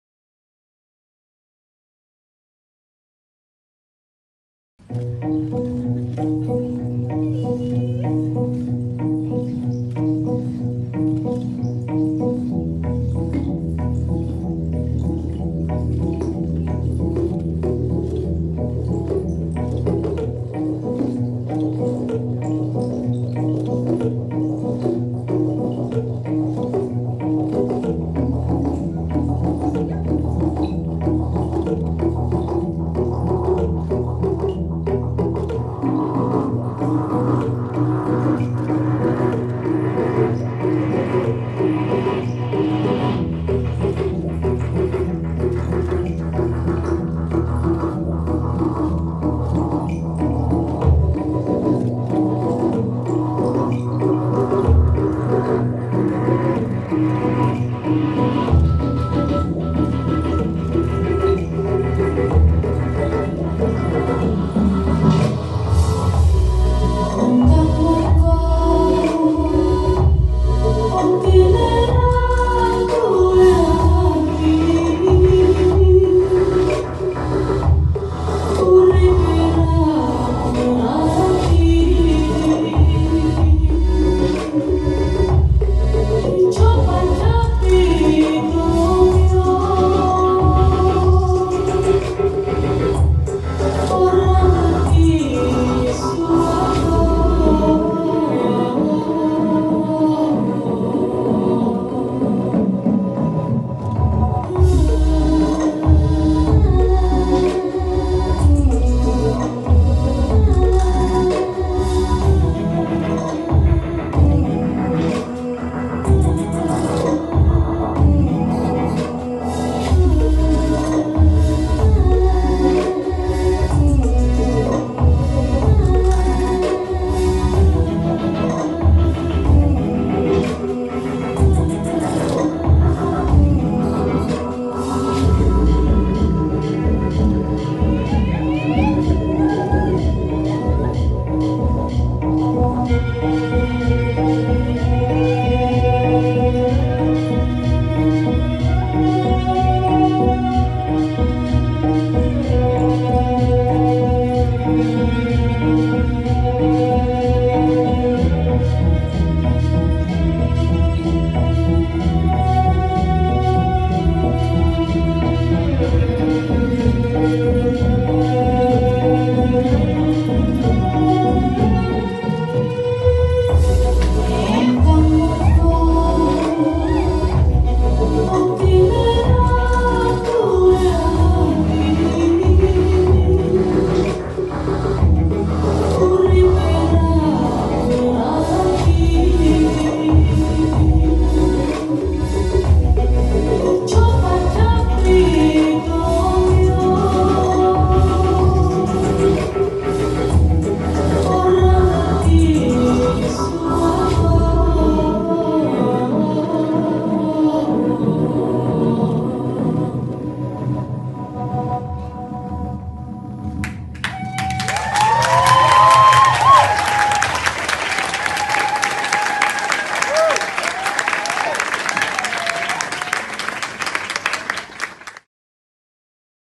Moona Khilana - Fusion Breeze 5 Show
Moona Khilana @ the show of Fusion Breeze V, the tribal and fusion bellydance festival in Ghent, hosted by Teuta.
Location: Planeet Mars, Gent, Belgium